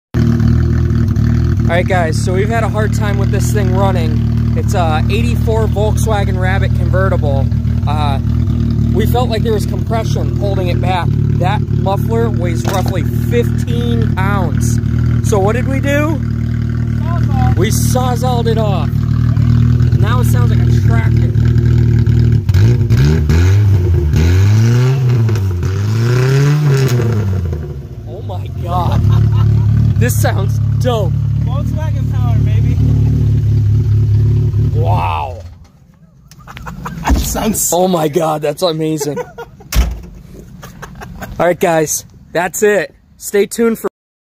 MK1 VW No Muffler! COMPLETELY FULL of ACORNS! 🌰
Muffler was full of acorns! weighed 15LBS! I guess it's not a surprise after 12 years in a field